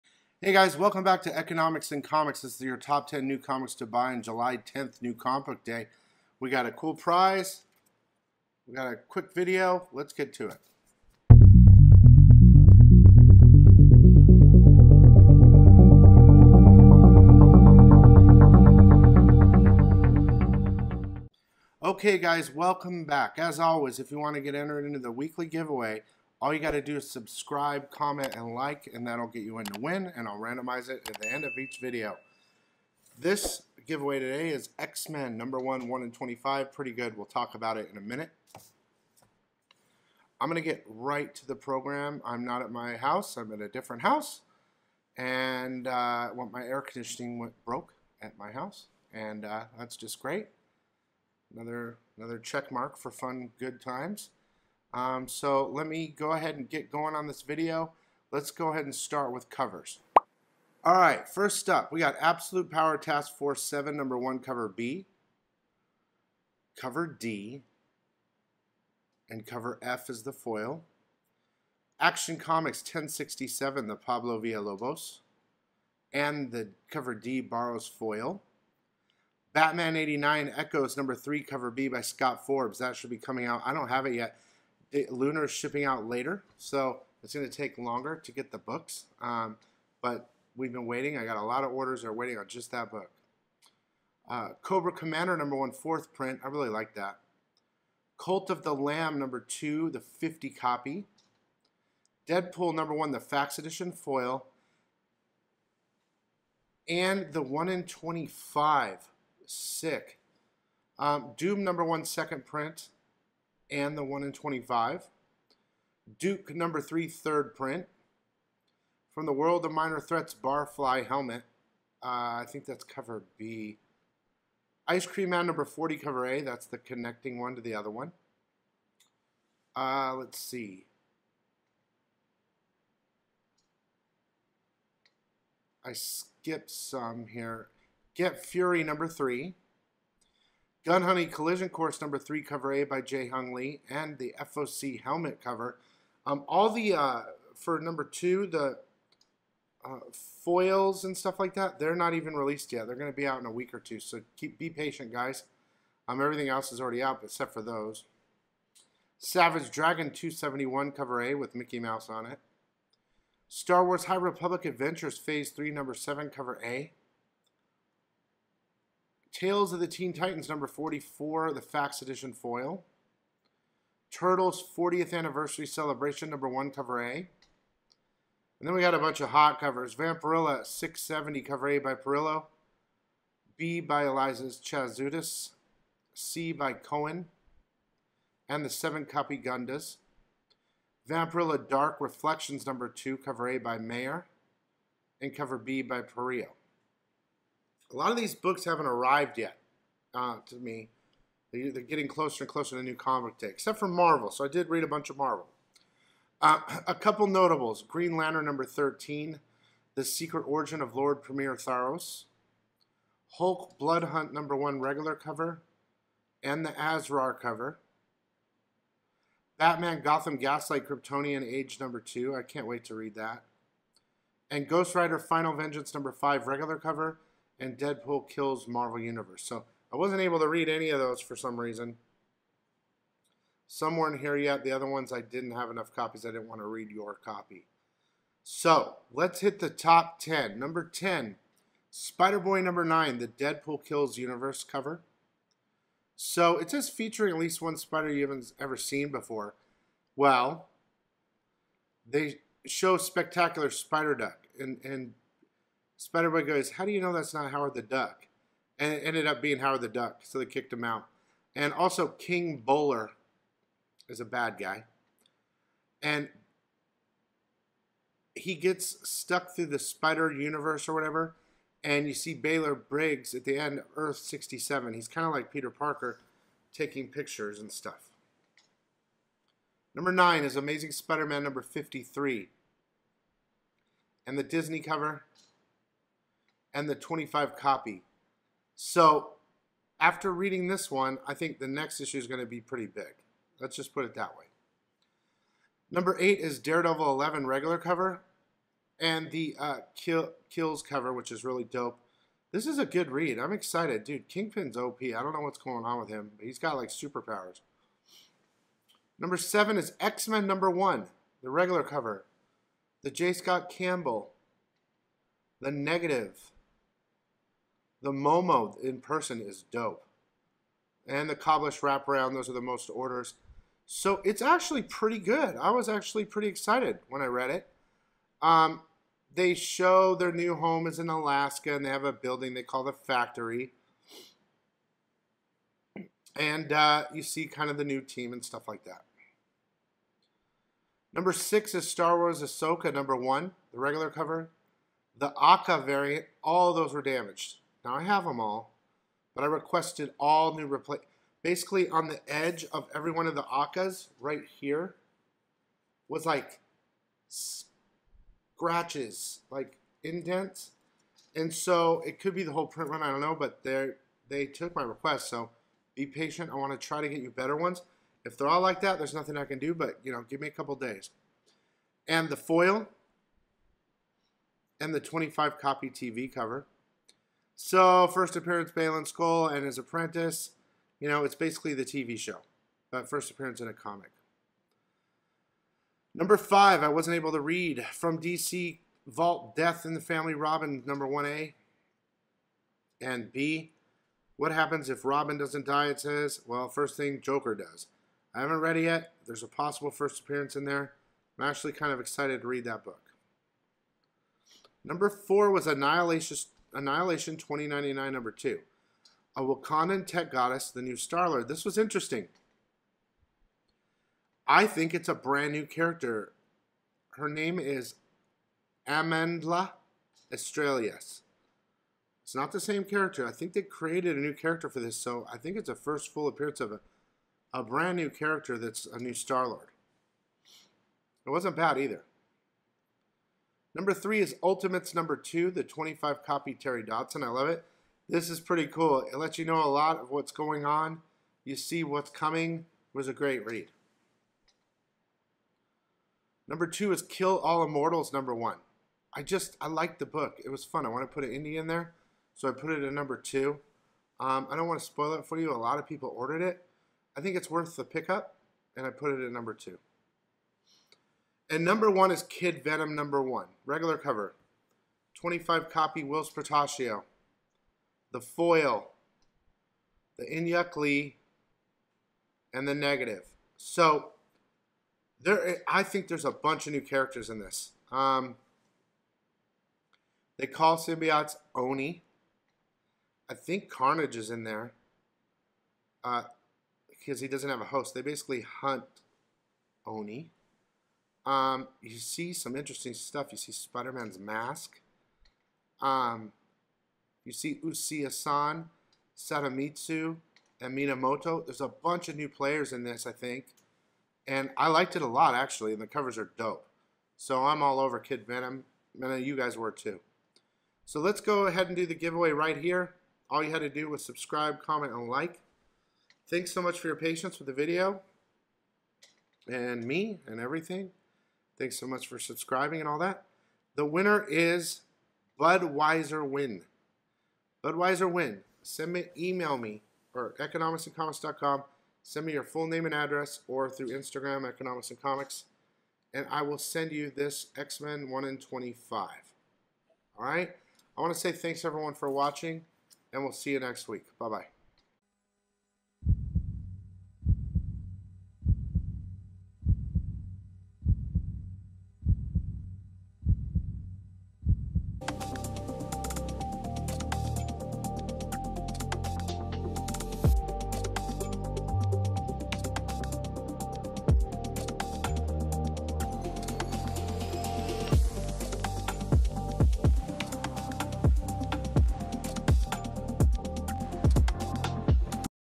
Top 10 New Comic Books July 10th 2024 🔥 Reviews, Covers, & Giveaway
Welcome to Economics In Comics.  Your home for up-to-date news and comic book information.